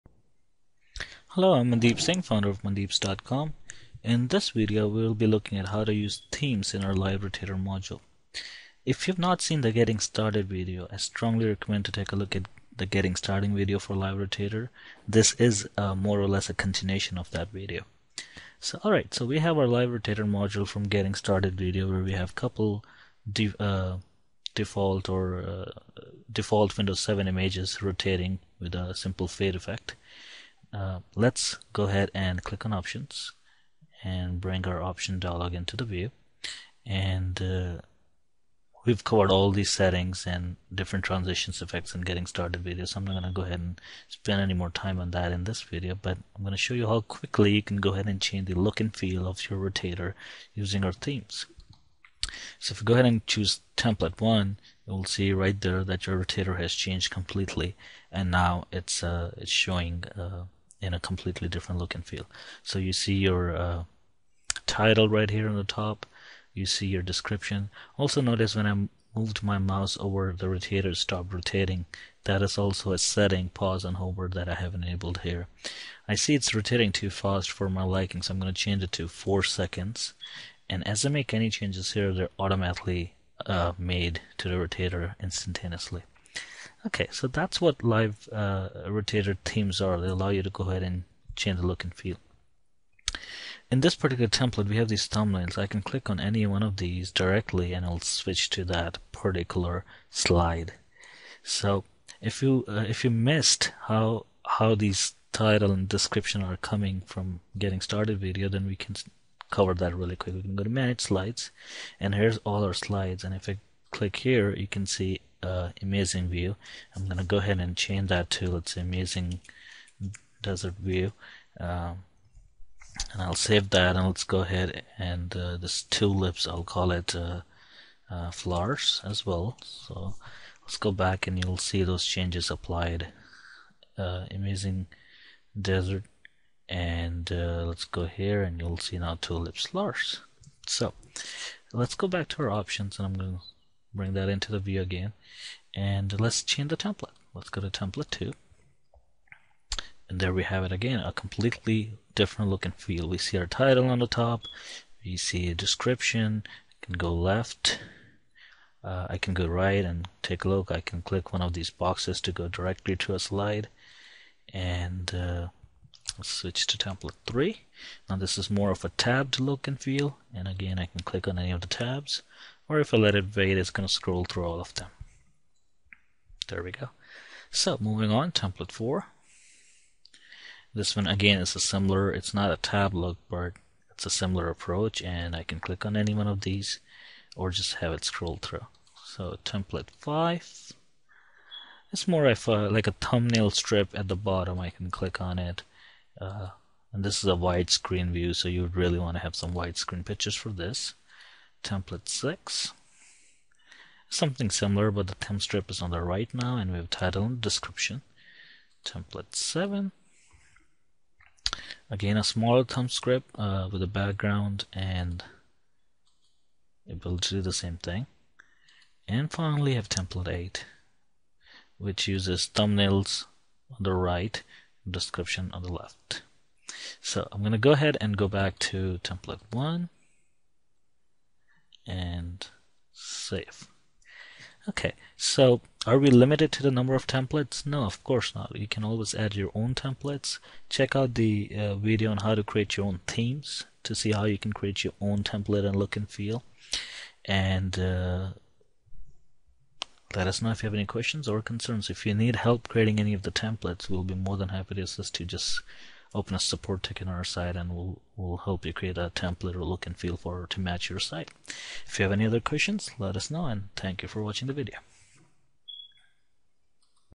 Live Rotator - Working w/Themes | DotNetNuke Module | Mandeeps.com
Learn how to work with themes in Live Rotator